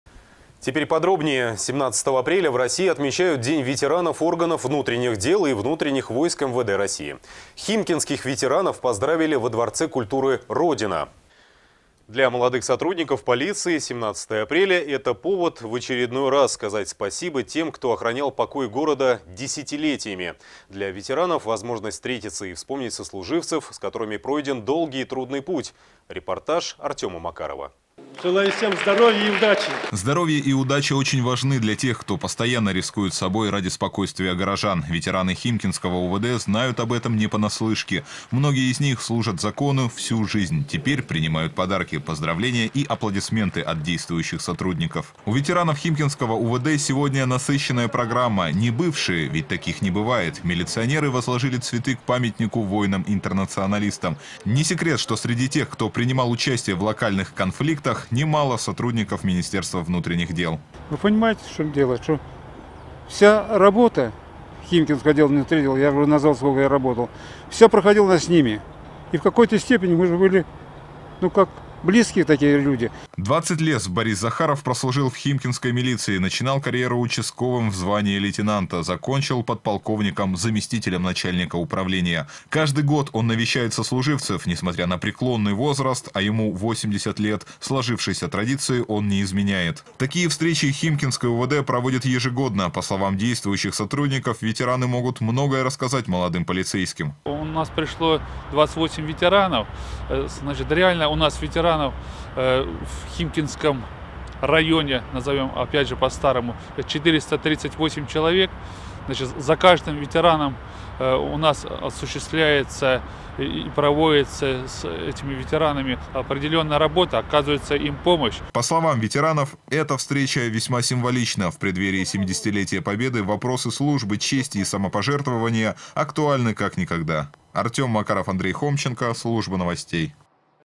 Химкинских ветеранов МВД поздравили во Дворце культуры «Родина»
17-го  апреля в России отмечают День ветеранов органов внутренних дел и внутренних войск МВД России. Для молодых сотрудников полиции этот день является поводом в очередной раз сказать «спасибо» тем, кто охранял покой города десятилетиями. Для ветеранов – это возможность встретиться и вспомнить сослуживцев, с которыми пройден долгий и трудный путь.